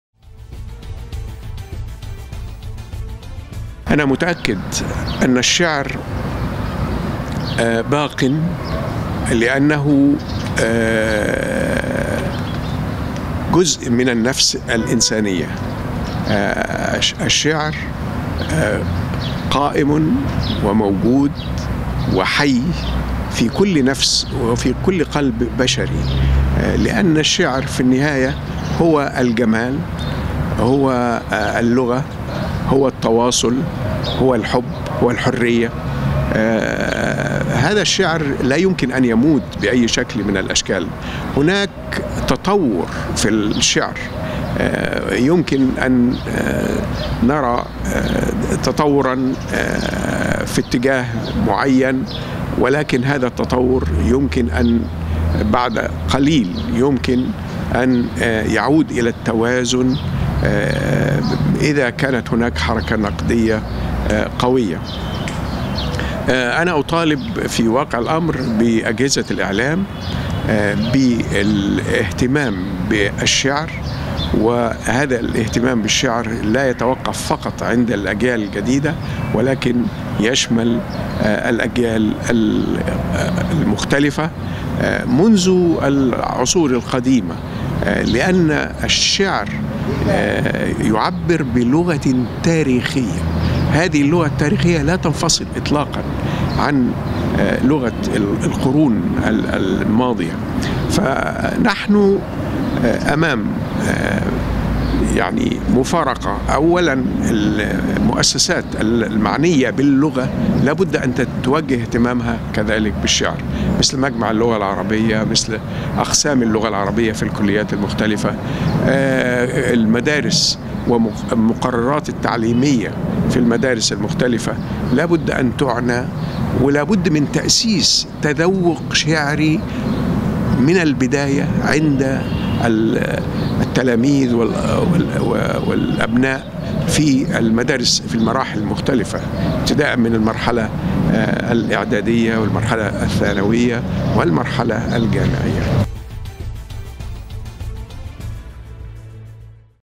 مصر العربية | في يومه العالمي.. مطالب الشاعر أبو سنة للاهتمام بالشعر
طالب الشاعر محمد إبراهيم أبو سنة بالعديد من المطالب للاهتمام بالشعر في الإعلام والمناهج الدراسية ومن وزارة الثقافة ، وذلك بالتزامن مع الاحتفال باليوم العالمي للشعر.